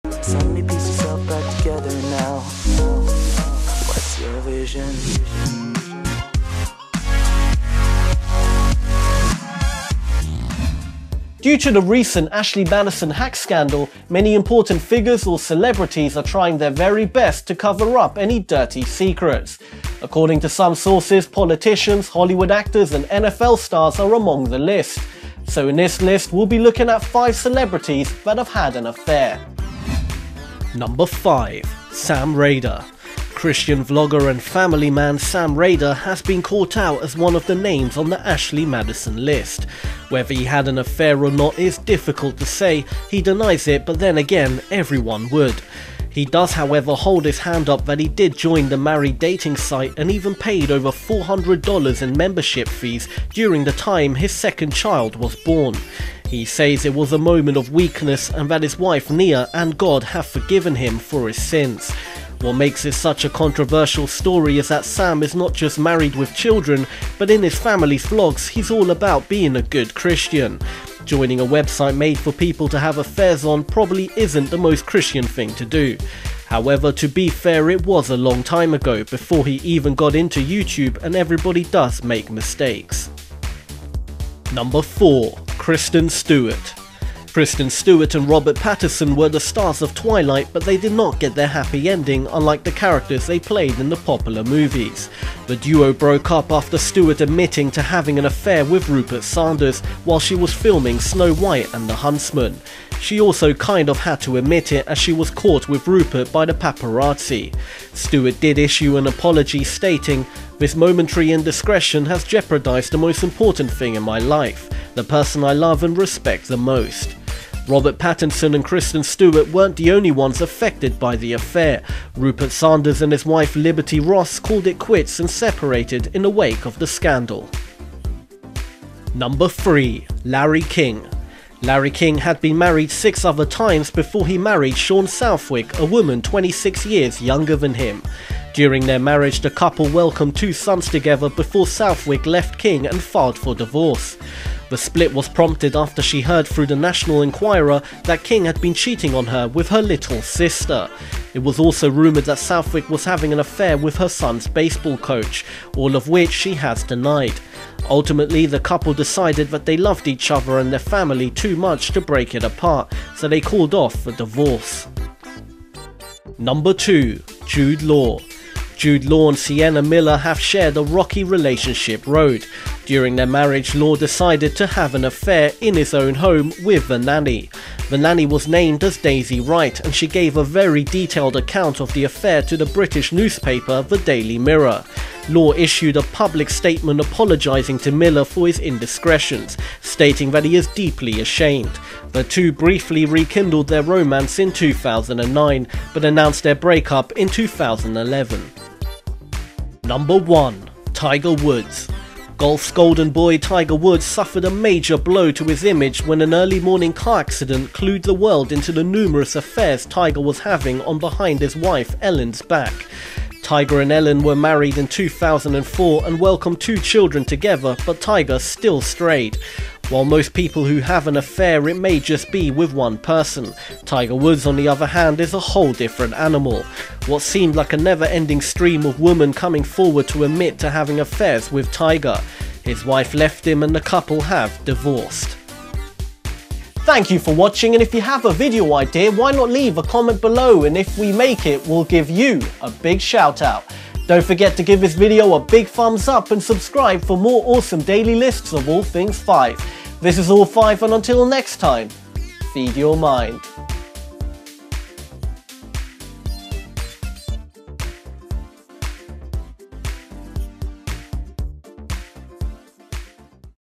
5 Celebrities That Had An Affair!
Due to the recent Ashley Madison hack scandal, many important figures or celebrities are trying their very best to cover up any dirty secrets.

According to some sources, politicians, Hollywood actors & NFL stars are among the list.

So in this list we'll be looking at 5 celebrities that have had an affair.

SECOND CHANNEL

Until Next Time... Feed Your Mind!

5) Sam Rader

Christian vlogger & family man, Sam Rader has been caught out as one of the names on the Ashley Madison list.

Whether he had an affair or not is difficult to say, he denies it but then again everyone would.

He does however hold his hands up that he did join the married dating site & even paid over $400 in membership fees, during the time his second child was born.

He says it was a moment of weakness & that his wife Nia & God have forgiven him for his sins.

What makes this such a controversial story is that Sam is not just married with children, but in his families vlogs hes all about being a good Christian.

Joining a website made for people to have affairs on, probably isn't the most Christian thing to do.

However to be fair it was a long time, before he even got into YouTube & everybody does make mistakes.

4) Kristen Stewart

Kristen Stewart & Robert Pattinson were the stars of Twilight but they did not get their happy ending, unlike the characters they played in the popular movies.

The duo broke up after Stewart admitted to having an affair with Rupert Sanders while she was filming “Snow White and the Huntsman.”

She also kind of had to admit it as she was caught with Rupert by the paparazzi.

Stewart did issue an apology stating “This momentary indiscretion has jeopardized the most important thing in my life, the person I love and respect the most, Rob. I love him, I love him, I’m so sorry.”

Robert Pattinson and Kristen Stewart weren't the only ones effected by the affair, Rupert Sanders and his wife Liberty Ross called it quits and separated in the wake of the scandal.

3) Larry King

Larry King had been married six other times before he married Shawn Southwick , a woman 26 years younger than him.

During their marriage the couple welcomed two sons together before Southwick left King and filed for divorce.

The split was prompted after she heard through the “National Enquirer” that King had been cheating on her with her little sister!

It was also rumored that Southwick was having an affair with her son’s baseball coach, all of which she has denied.

Ultimately, the couple decided that they loved each other and their family too much to break it apart, so they called off the divorce.

2) Jude Law

Jude Law and Sienna Miller have shared a rocky relationship road.

During their marriage Law decided to have an affair in his own home, with the nanny.

The nanny was named as Daisy Wright & she gave a very detailed account of the affair to the British newspaper the Daily Mirror.

Law issued a public statement apologizing to Miller for his indiscretions, stating that he is “deeply ashamed.”

The two briefly rekindled their romance in 2009 but announced their break up in 2011.

1) Tiger Woods

Golf’s golden boy Tiger Woods suffered a major blow to his image when an early morning car accident clued the world into the numerous affairs Tiger was carrying on behind his wife Elin’s back.

Tiger and Elin were married in 2004 and welcomed two children together but Tiger still strayed.

While most people who have an affair it may just be with 1 person, Tiger Woods on the other hand is a whole different animal.

What seemed like a never-ending stream of women came forward to admit to having affairs with Tiger.

His wife left him & the couple have divorced.